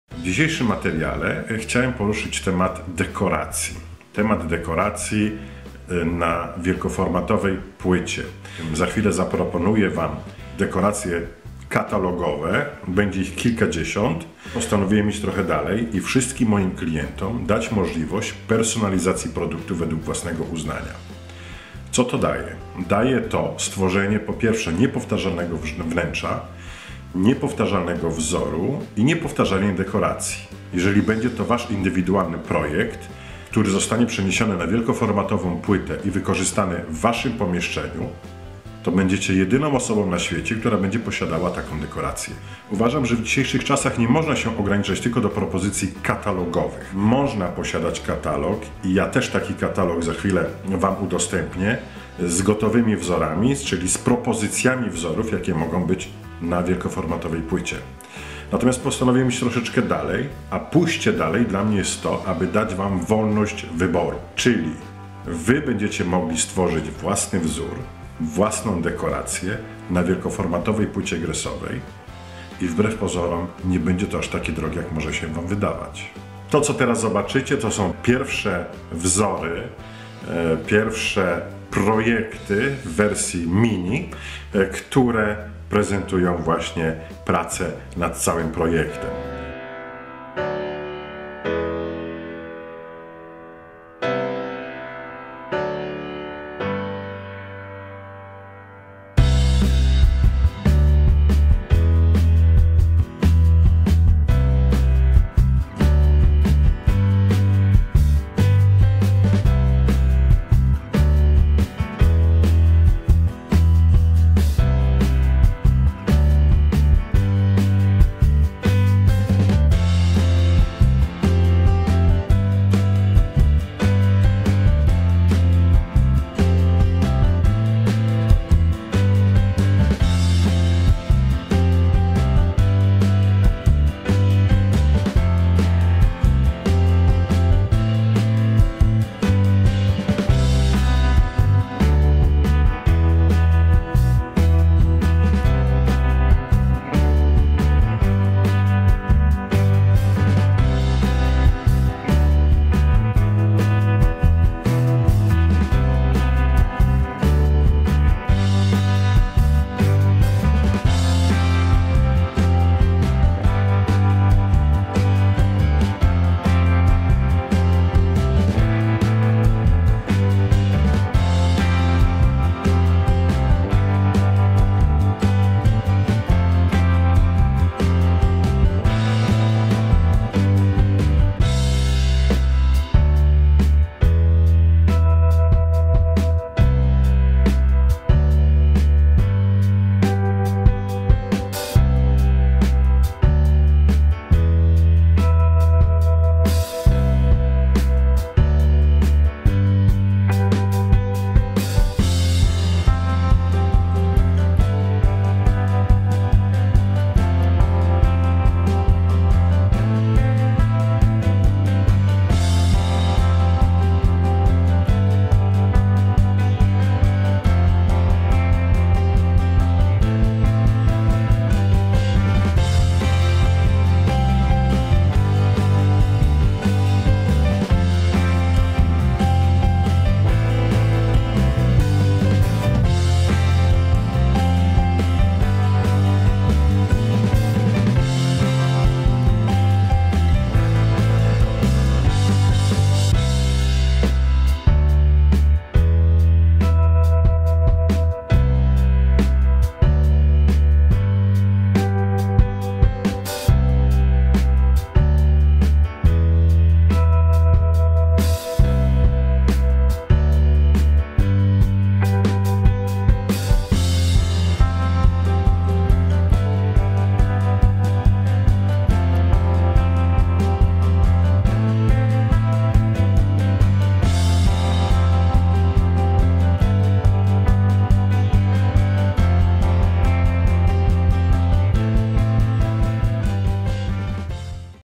Wielkoformatowe płyty gresowe według twojego projektu (spieki kwarcowe)
Dzisiaj prezentują absolutną nowość! Jeśli jesteś architektem, projektantem wnętrz dekoratorem lub klientem indywidualnym, który nie zadowala się proponowanymi rozwiązaniami dekoracyjnymi, to mam dla ciebie dobrą wiadomość. Dzięki najnowszej technologii cyfrowej stworzę dekorację na wielkiej płycie gresowej tzw. spieku kwarcowym o wymiarze 120x260cm lub 100x300 według twojego projektu. Łamię bariery, które wprowadzili producenci oferują ograniczone możliwości w tworzeniu niepowtarzalnych wnętrz. Możesz stworzyć wyjątkowe unikalne na skalę światową wnętrze, które będzie posiadało twoje dekoracje.